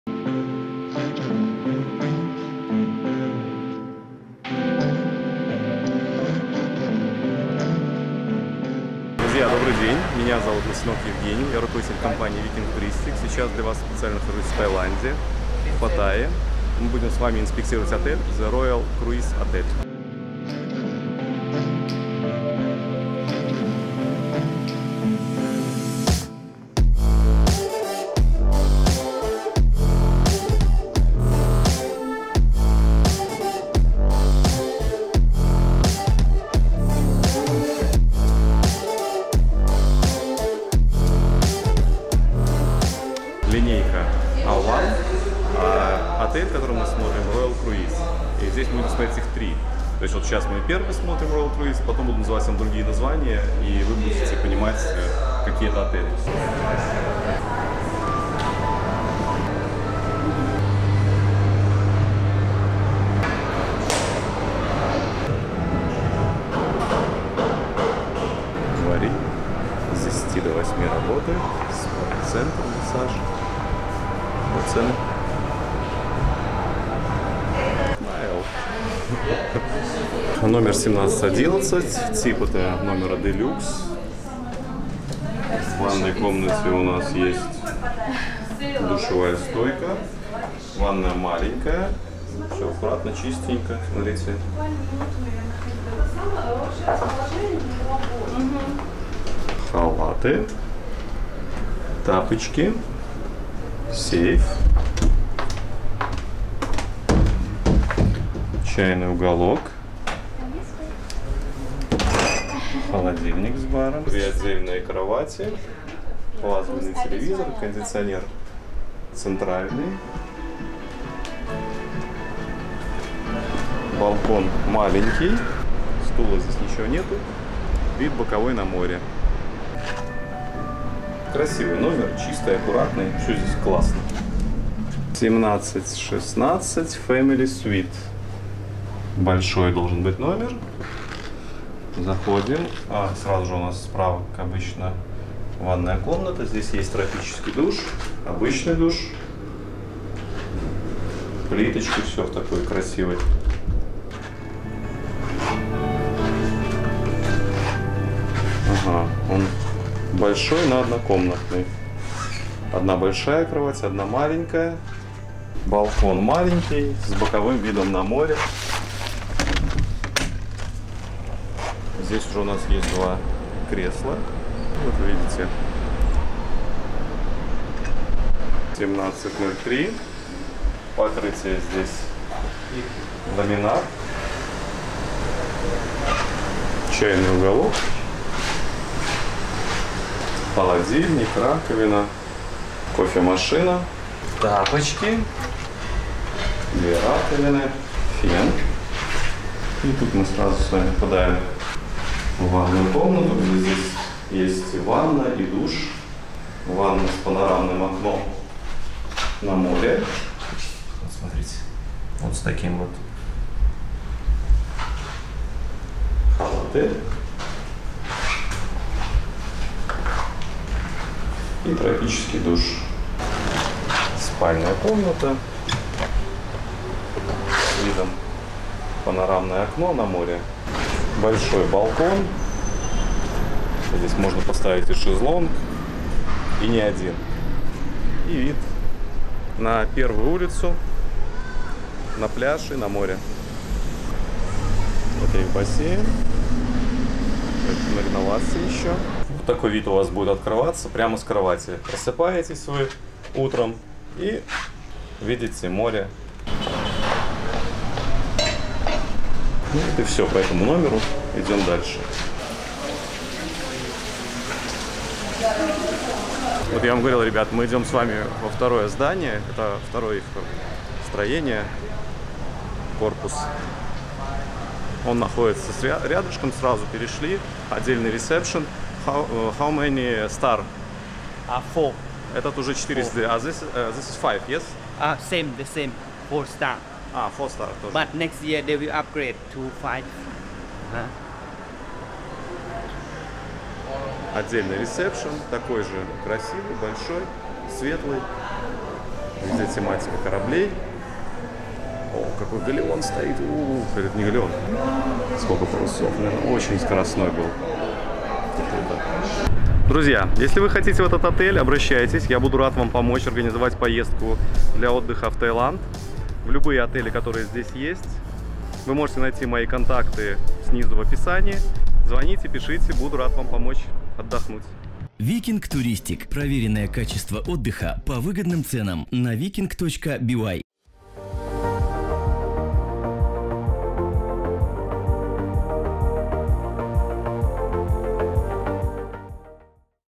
Каким будет отпуск в тайской четверке? Смотрим городской отель A One The Royal Cruise Hotel. 4K
Краткое описание видео, общие впечатления

Обзор отеля A One The Royal Cruise Hotel:
00:20 Расположение отеля, расстояние от аэропорта до отеля
00:29 Ресепшен отеля
01:10 Спа салон
01:22 Номер Делюкс с боковым видом на море
02:15 Большой семейный номер
03:00 Номер с видом на море
04:00 Территория отеля, бассейн
04:32 Второй корпус отеля
05:09 Контакты для связи